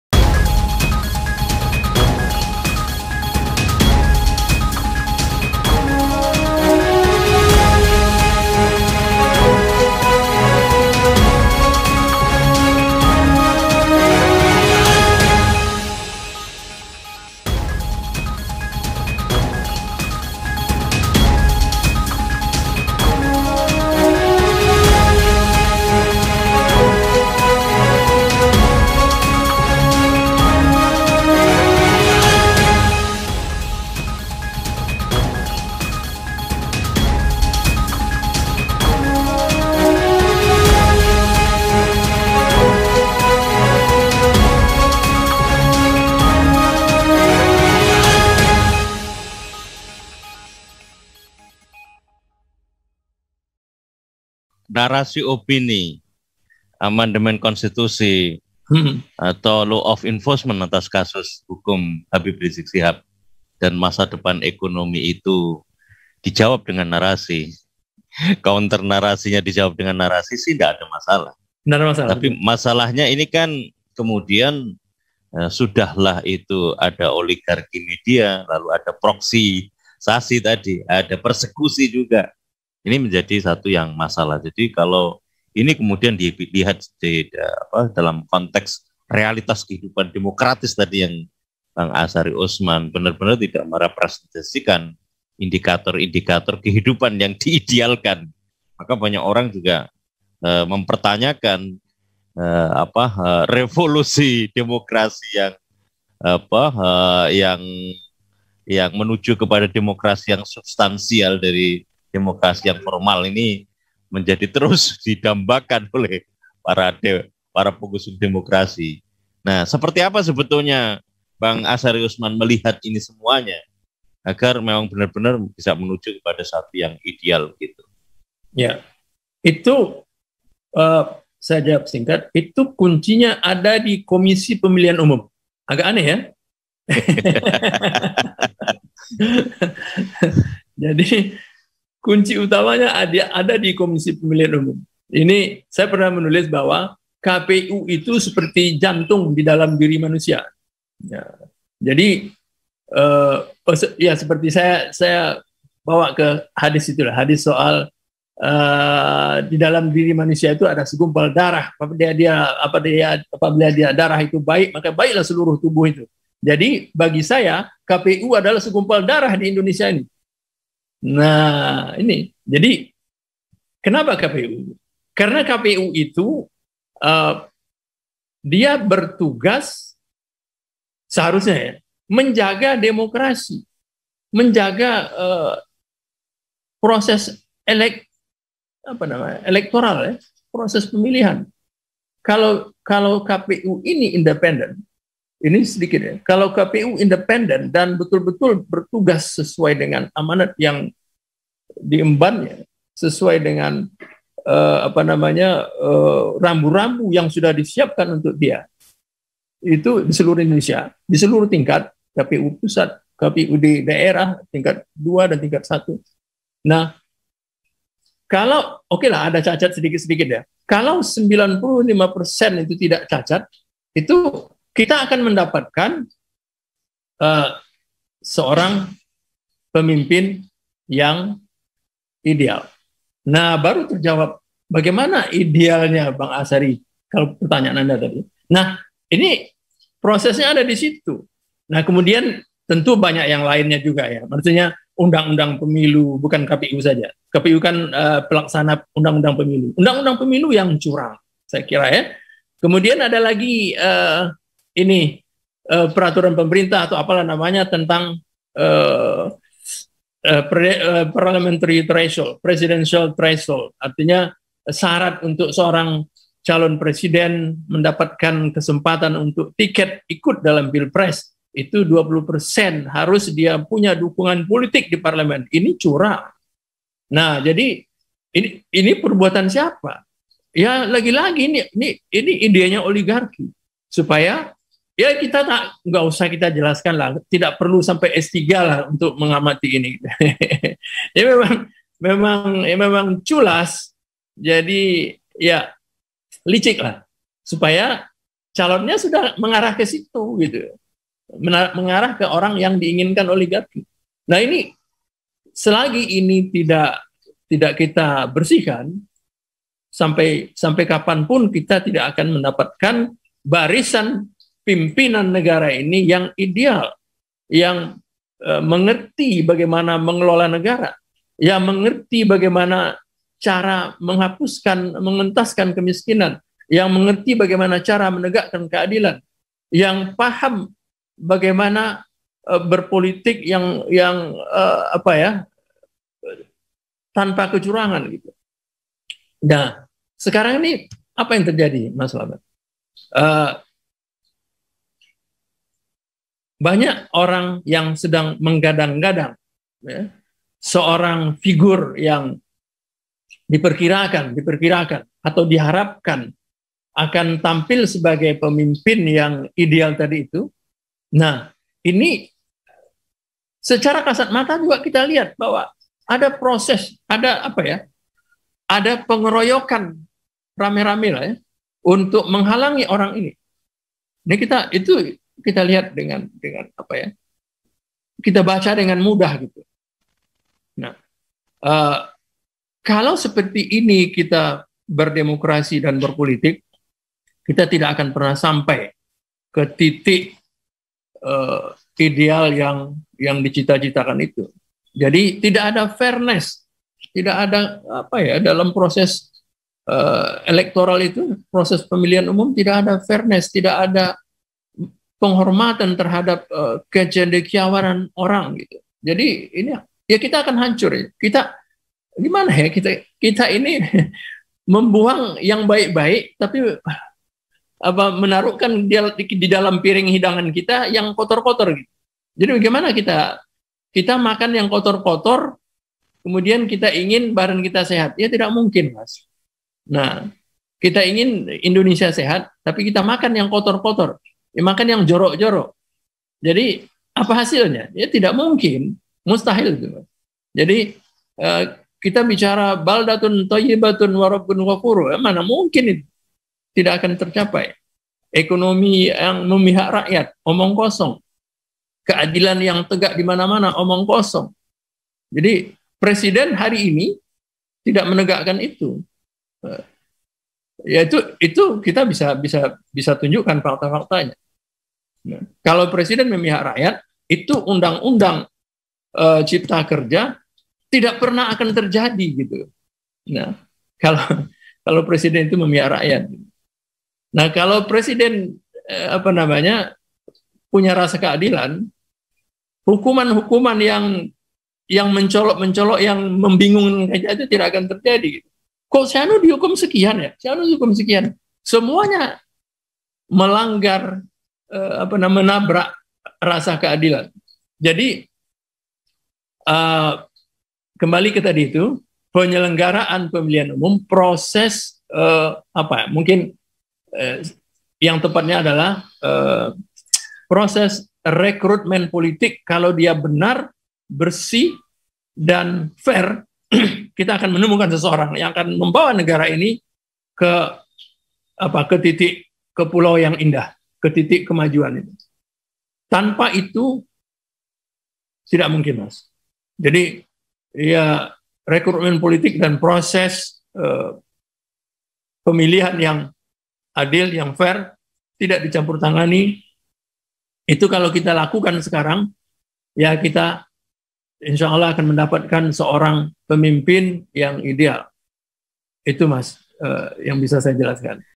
FGD #32 - Sesi Tanya Jawab Asyari Usman
🔴 [LIVE] FGD #32 Pusat Kajian Dan Analisis Data

Amandemen konstitusi, vonis hakim plagiarisme atas HAERES, dan masa depan ekonomi dalam bayang bayang krisis dan pandemi adalah tiga persoalan menonjol kekinian. Seolah menguak sengkarut persoalan ekonomi, hukum dan politik sekaligus. Akankah masa depan Indonesia bisa diwujudkan lebih baik dengan pengelolaan tata pemerintahan dalam sistem politik eksisting. Di tengah amburadulnya penegakan hukum yang cenderung banyak menabrak prinsip equility before the law penuh aroma politik. Upaya untuk melanggengkan kekuasaan melalui pintu amandemen konstitusi sangat menyayat nurani. Dan euforia pertumbuhan ekonomi yang masih perlu diuji dan diverifikasi. Apa dan bagaimana memahami dan mensikapinya ? Mari hadir dan ikuti diskusinya pada :

🗓️ Hari/Tanggal : Sabtu, 11 September 2021
⏰ Waktu : 08.00 sd 11.30 WIB
🎞️ Tema : Amandemen Konstitusi, Plagiarisme Hukum HAERES dan Masa Depan Ekonomi

🎙️ Keynote Speaker : Fajar Kurniawan - Analis Senior PKAD

📞 Pembicara :
1) Ustadz H.M. Ismail Yusanto - Cendekiawan Muslim
2) Ustadz Dwi Tjondro Triono Ph.D. - Ekonom Syariah
3) Dr. Abdul Chair Ramadhan S.H., M.H. - HRS Centre
4) Dr. Ahmad Yani S.H., M.H. - Partai Masyumi
5) Dr. Refly Harun S.H., M.H. - Pakar Hukum Tata Negara
6) Asyari Usman - Wartawan Senior

Passcode: FGD#31

Pusat Kajian Dan Analisis Data